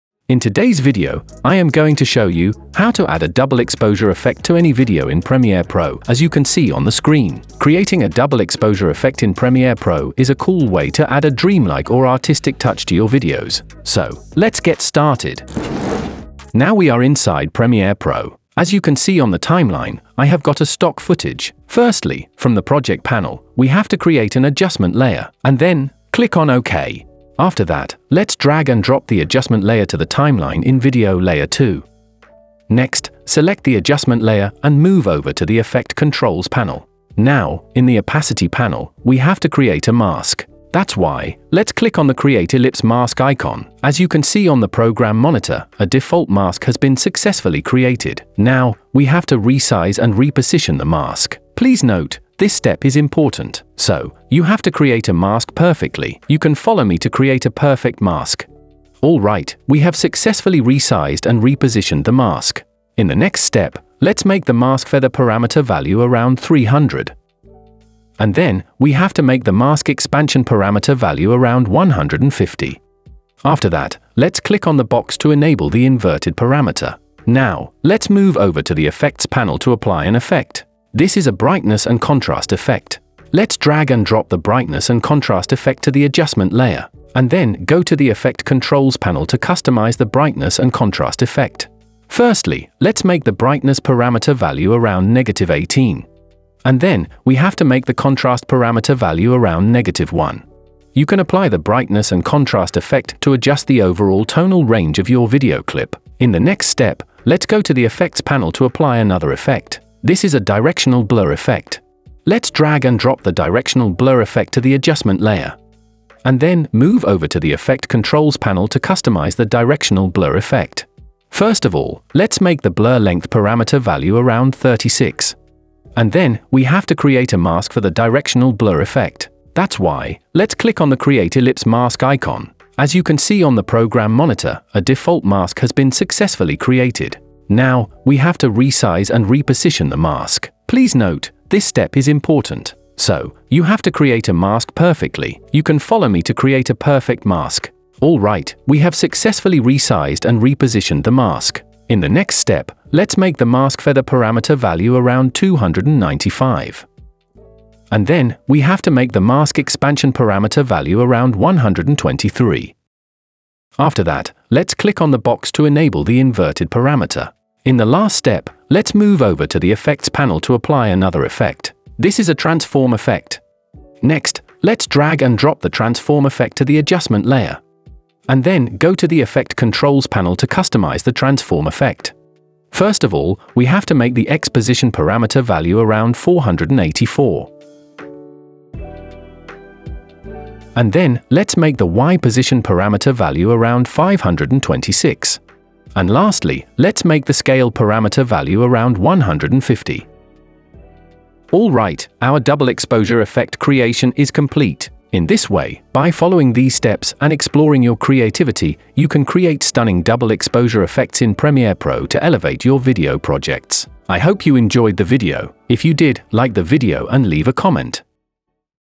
ADD Double Exposure Effect to VIDEO in Adobe Premiere Pro
Experience the captivating Double Exposure Effect on your video using Adobe Premiere Pro. Elevate your content with this stunning visual enhancement.